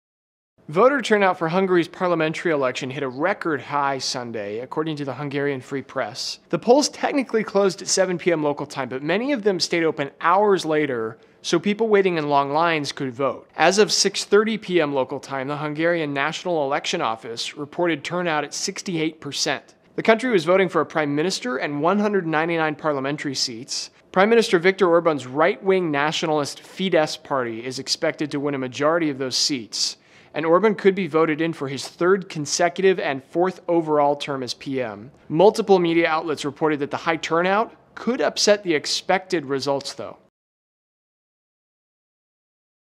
Hungary's voter turnout hits record high
Multiple media outlets report that the high turnout could upset the expected results.